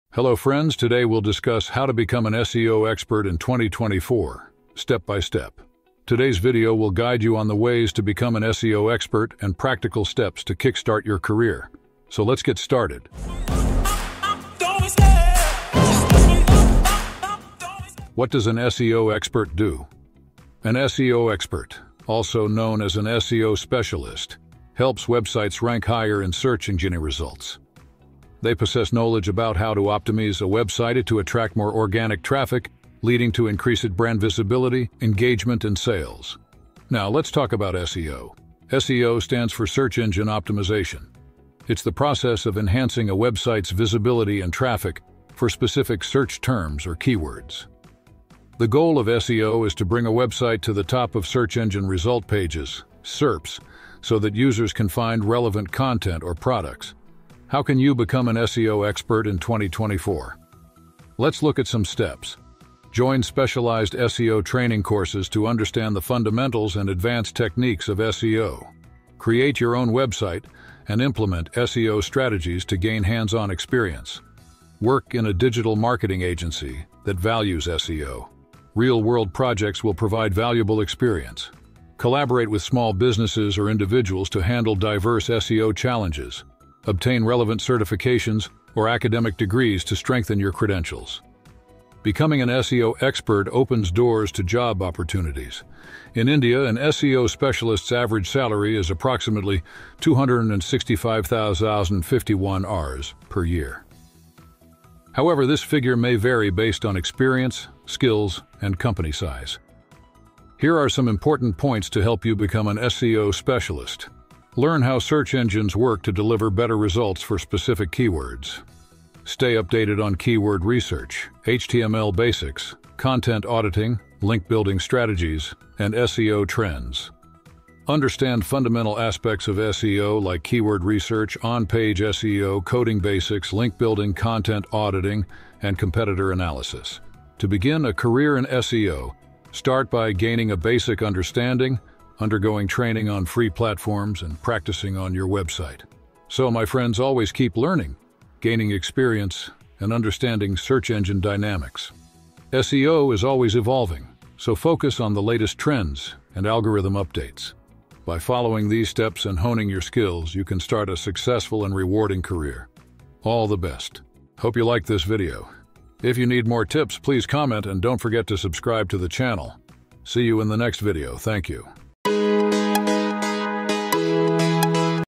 How to become SEO Expert in 2024 ? | Step -by- Step | SEO Tutorial
Welcome to our comprehensive guide on mastering SEO in 2024! In this video, we delve into the essential steps and strategies to become an SEO expert and kickstart a successful career in this dynamic field.

🔍 What's Covered in This Video:

Understanding the Crucial Role of an SEO Expert
Decoding the World of SEO: Search Engine Optimization Explained
Step-by-Step Guide: How to Become an SEO Expert in 2024
Insights into SEO Career Prospects and Salaries in India
Important Keys and Skills Required to Excel in SEO
Tips to Begin Your Journey Towards a Rewarding Career in SEO
In this tutorial, we explore everything from specialized training courses to practical hands-on experiences, all designed to equip you with the knowledge and skills needed to thrive as an SEO specialist in 2024.

🌟 Key Takeaways:

Learn the fundamentals and advanced techniques of SEO.
Gain hands-on experience by optimizing your own website.
Understand the career prospects and salary insights in the SEO industry.
Discover crucial skills and tips to start your SEO journey effectively.
Whether you're new to SEO or seeking to upgrade your skills, this video provides invaluable insights to help you navigate the ever-evolving landscape of search engine optimization in 2024.